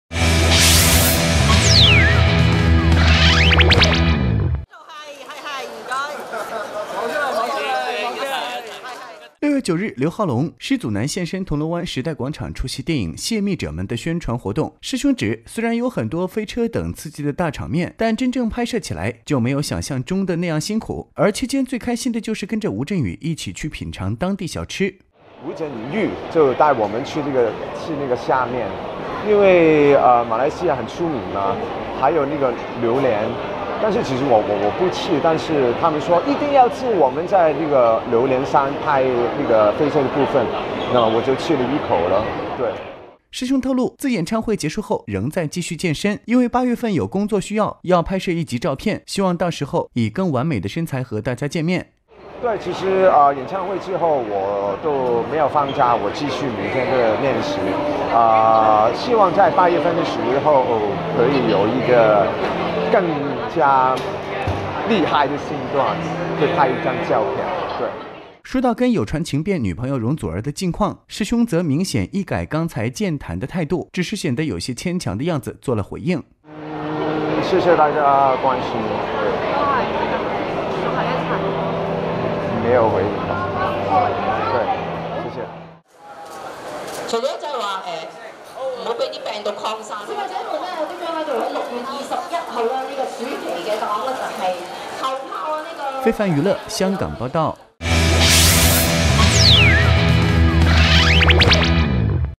劉浩龍為拍攝狂練好身材 談及與容祖兒感情事拒絕回應
6月9日，劉浩龍（師兄）、施祖男現身銅鑼灣時代廣場出席電影《洩密者們 》的宣傳活動。師兄指雖然有很多飛車等刺激的大場面，但真正拍攝起來就沒有想像中的那樣辛苦，而期間最開心的就是跟著吳鎮宇一起去品嘗當地小吃。